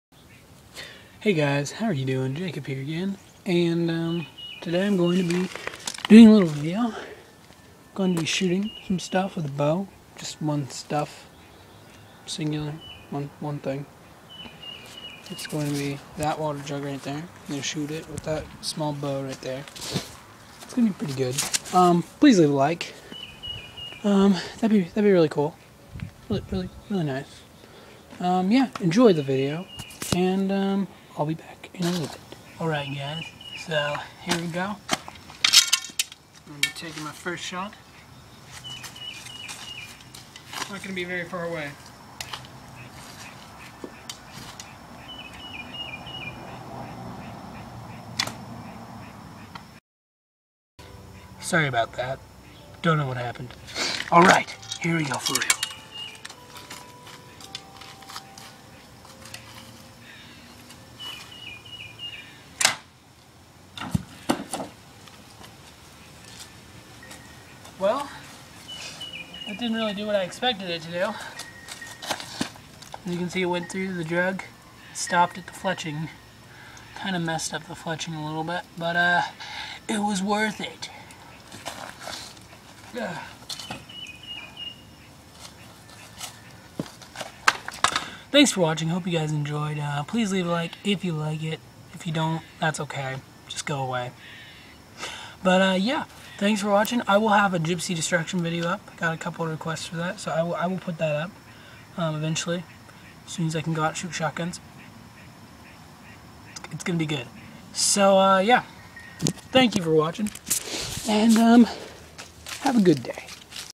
Shooting my bow at a jug of water!!
as soon as I can go out and shoot shot guns ill make that gypsy destruction video!